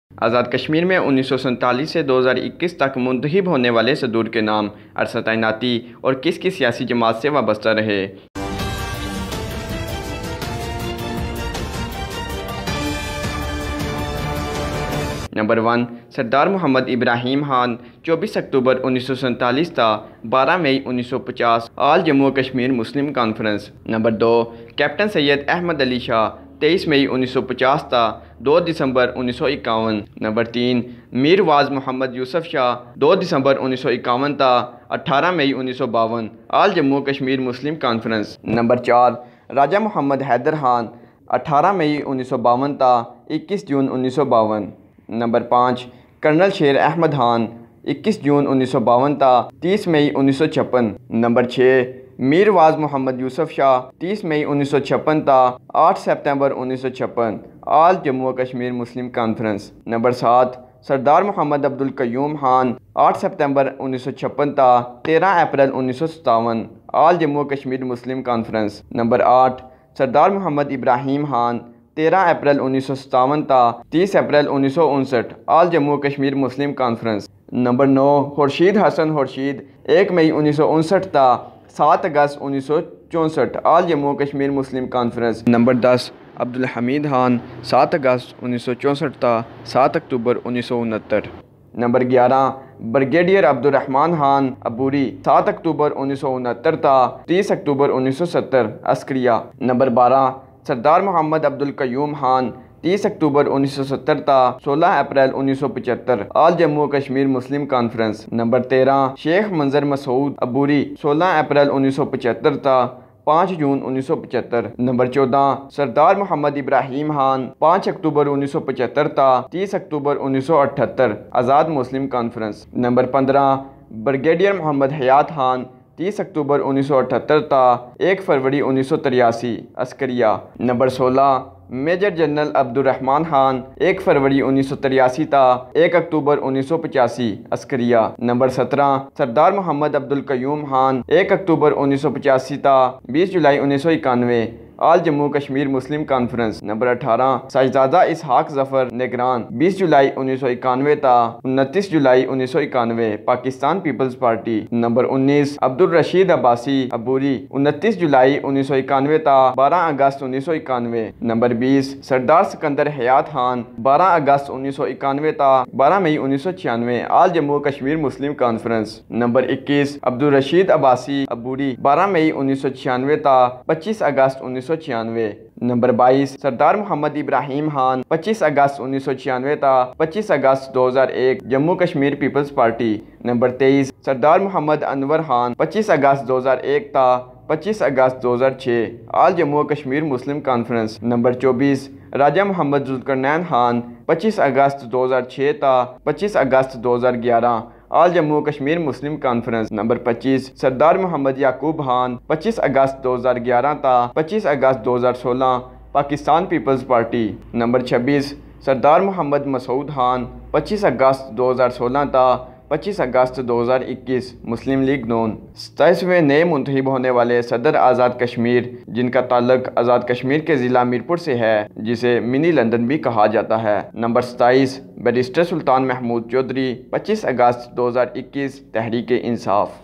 k7 - Detail of Presidents Azad Kashmir
1 Sardar Muhammad Ibrahim Khan 24 October 1947 12 May 1950 All Jammu and Kashmir Muslim Conference
2 Captain General Syed Ali Ahmed Shah 30 May 1950 2 Dec 1951 Military
3 Mirwaiz Yusuf Shah 2 Dec 1951 18 May 1952 All Jammu and Kashmir Muslim Conference
[1] Raja Muhammad Haydar Khan (interim) 18 May 1952 21 Jun 1952 -
4 Colonel Sher Ahmed Khan 21 Jun 1952 30 May 1956 Military
(3) Mirwaiz Yusuf Shah 30 May 1956 8 Sep 1956 All Jammu and Kashmir Muslim Conference
5 Sardar Mohammad Abdul Qayyum Khan 8 Sep 1956 13 Apr 1957 All Jammu and Kashmir Muslim Conference
(1) Sardar Muhammad Ibrahim Khan 13 Apr 1957 30 Apr 1959 All Jammu and Kashmir Muslim Conference
6 Khurshid Hasan Khurshid (First Elected President) 1 May 1959 7 Aug 1964 Jammu and Kashmir Liberation League
7 Abdul Hamid Khan 7 Aug 1964 7 Oct 1969 -
[2] Brigadier Abdul Rahman Khan (interim) 7 Oct 1969 30 Oct 1970 Military
(5) Sardar Mohammad Abdul Qayyum Khan 30 Oct 1970 16 Apr 1975 All Jammu and Kashmir Muslim Conference
[3] Sheikh Manzar Masaud (interim) 16 Apr 1975 5 Jun 1975 -
(1) Sardar Muhammad Ibrahim Khan 5 Jun 1975 30 Oct 1978 Azad Muslim Conference
8 Brigadier Muhammad Hayat Khan 30 Oct 1978 1 Feb 1983 Military
9 Major General Abdul Rahman Khan 1 Feb 1983 1 Oct 1985 Military
(5) Sardar Mohammad Abdul Qayyum Khan 1 Oct 1985 20 Jul 1991 All Jammu and Kashmir Muslim Conference
10 Sahibzada Muhammad Ishaq Zaffar 20 Jul 1991 29 Jul 1991 Pakistan Peoples Party
4 Abdul Rashid Abbasi (interim) 29 Jul 1991 12 Aug 1991 -
11 Sardar Sikandar Hayat Khan 12 Aug 1991 12 May 1996 All Jammu and Kashmir Muslim Conference
[4] Abdul Rashid Abbasi (interim) 12 May 1996 25 Aug 1996 -
(1) Sardar Muhammad Ibrahim Khan 25 Aug 1996 25 Aug 2001 Jammu Kashmir Peoples Party
12 Sardar Muhammad Anwar Khan 25 Aug 2001 25 Aug 2006 All Jammu and Kashmir Muslim Conference
13 Raja Muhammad Zulqarnain Khan 25 Aug 2006 25 Aug 2011 All Jammu and Kashmir Muslim Conference
14 Sardar Muhammad Yaqoob Khan 25 Aug 2011 25 Aug 2016 Pakistan Peoples Party
15 Masood Khan 25 Aug 2016 24 Aug 2021 Pakistan Muslim League (N)
16 Sultan Mehmood Chaudhry 25 Aug 2021[4] Present Pakistan Tehreek-e-Insaf